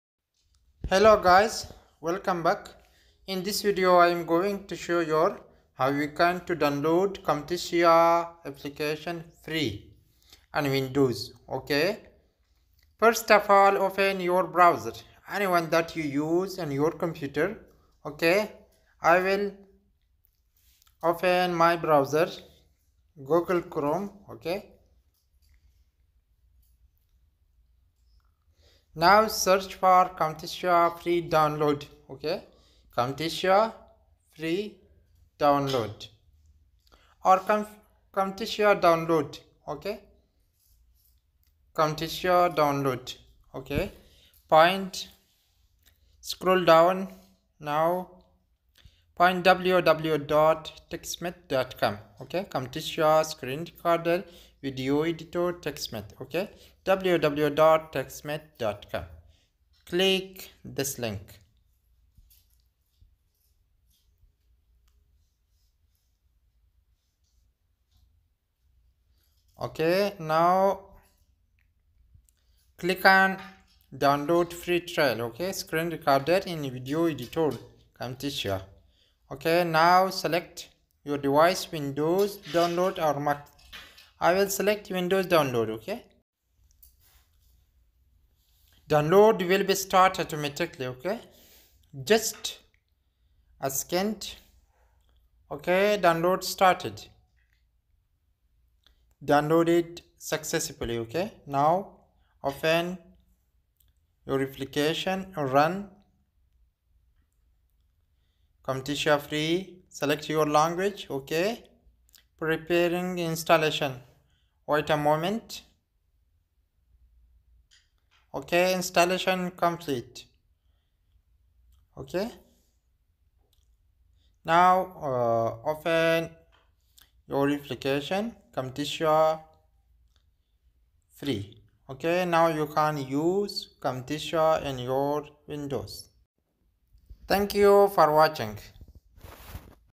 How to Download and install Camtasia for free Camtasia 2022
Hi Dears!
in This video i am  Showing to Your How We can To Download and install Camtasia Screen Recorder and Video Editor for free on Windows

i will be Create More Positive Videos About All Part of Coding,Developing and All Part of Social Media.